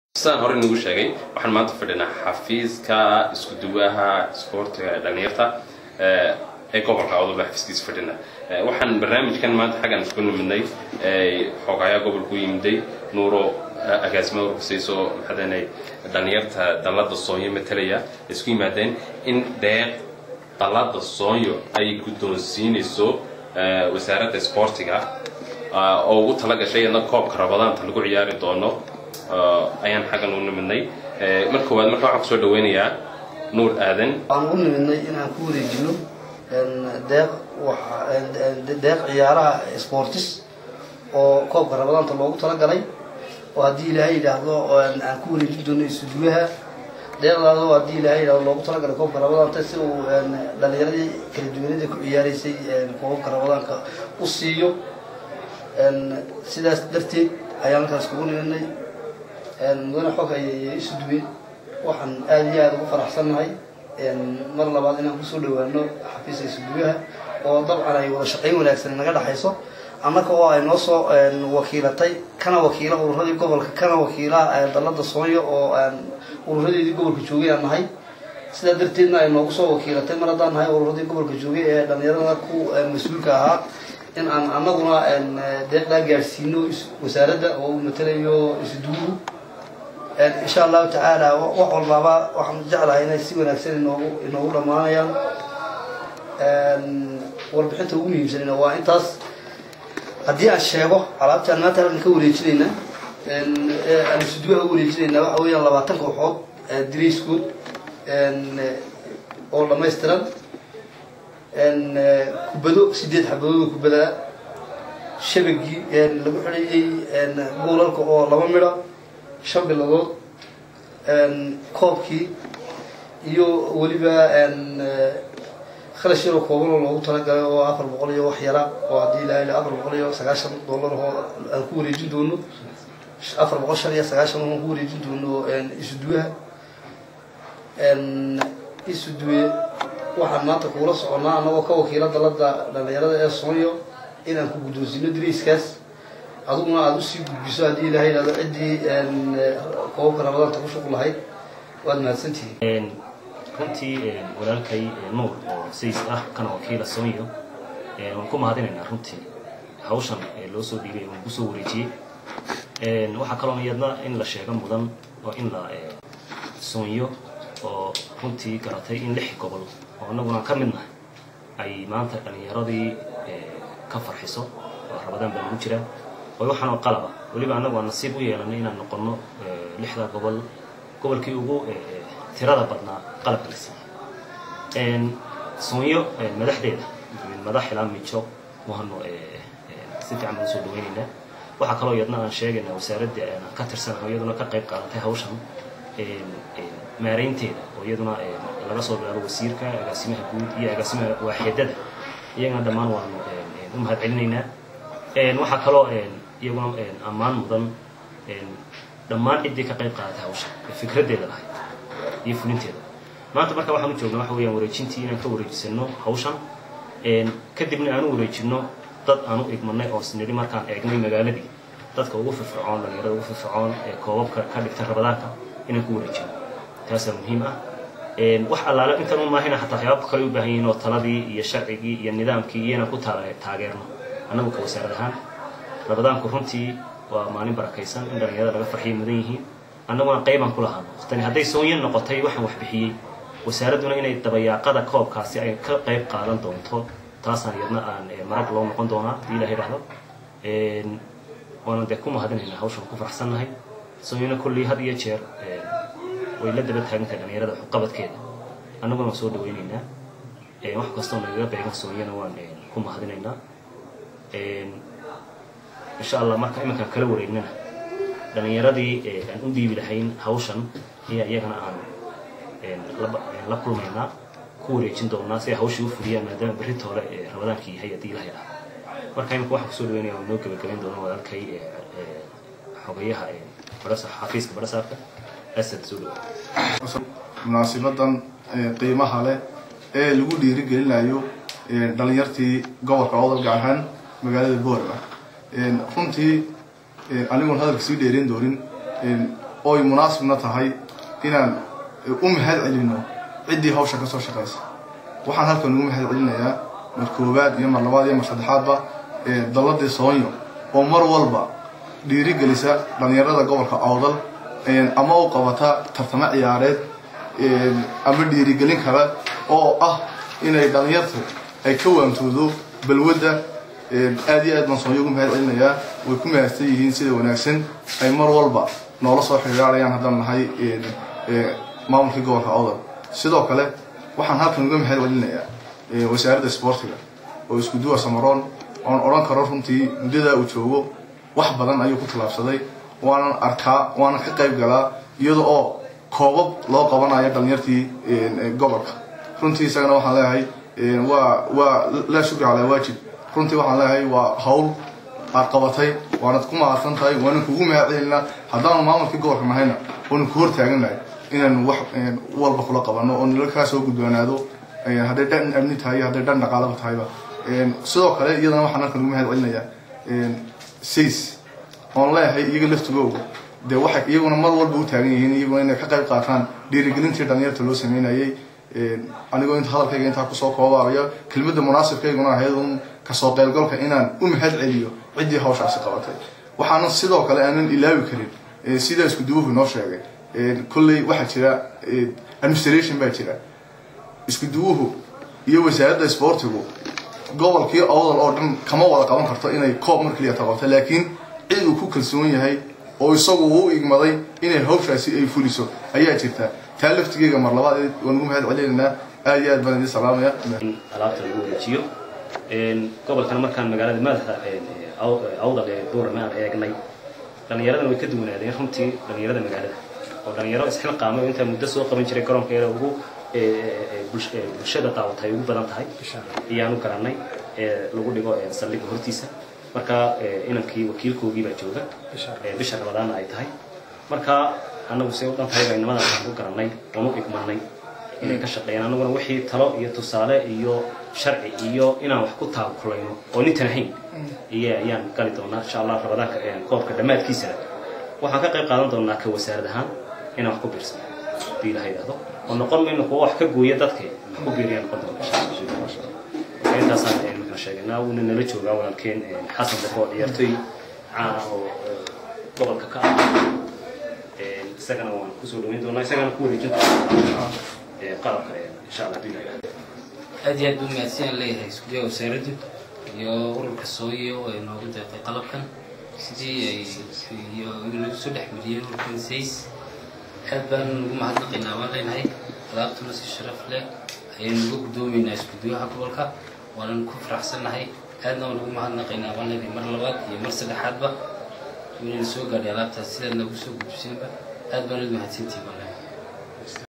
HADH Dalada SONYO Oo Agab Ciyaareed Ugu Deeqday tartanka Bisha Ramadan Ee Borama
Dalada SONYO Oo Agab Ciyaareed Ugu Deeqday tartanka Bisha Ramadaan Ee Boorama